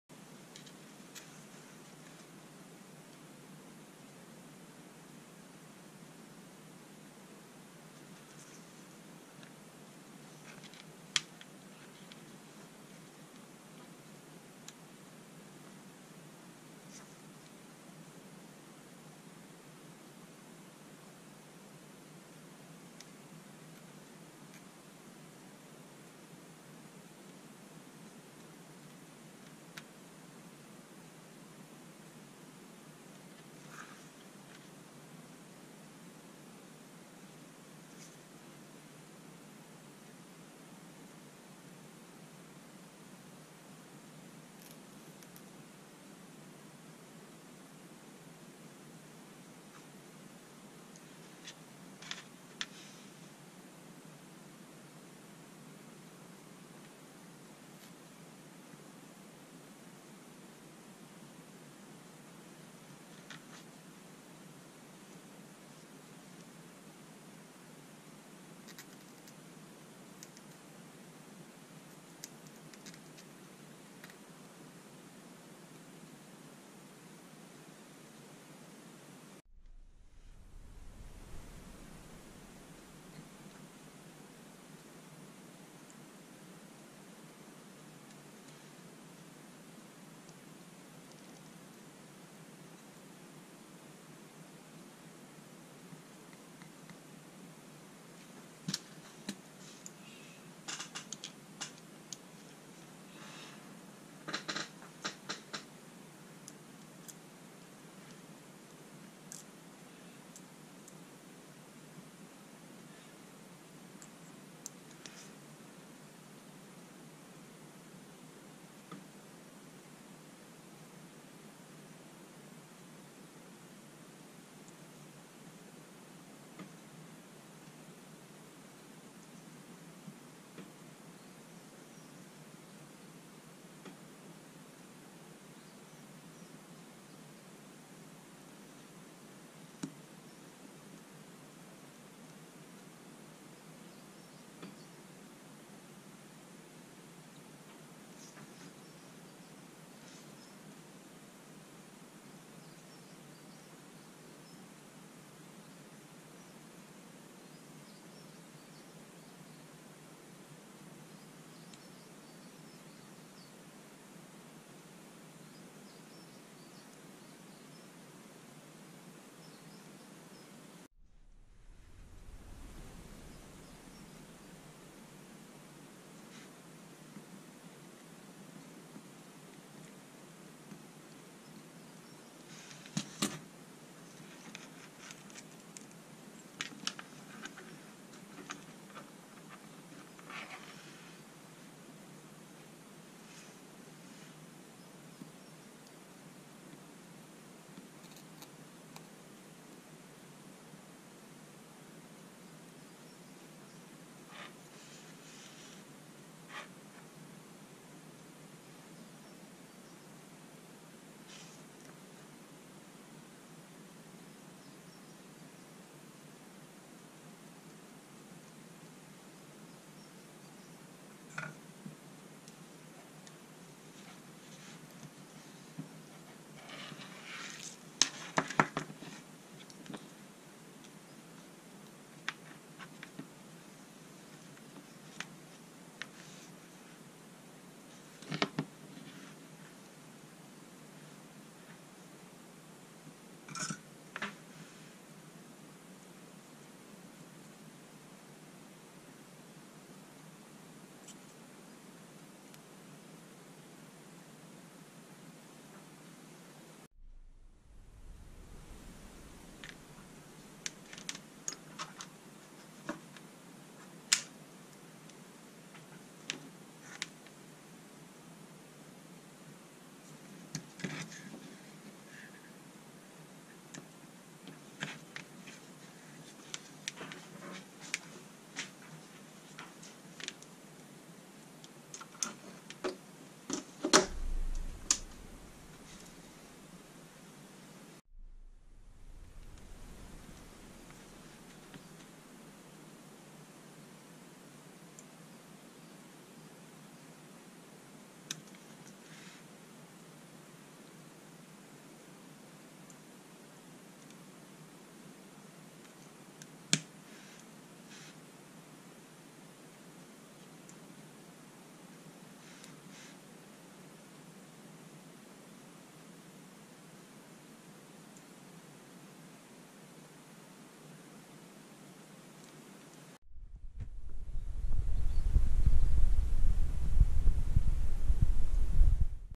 2S BMS with discharge protection
In this video I show how I wire a 2S BMS circuit with discharge protection
for two 18650 cells in series. (2S).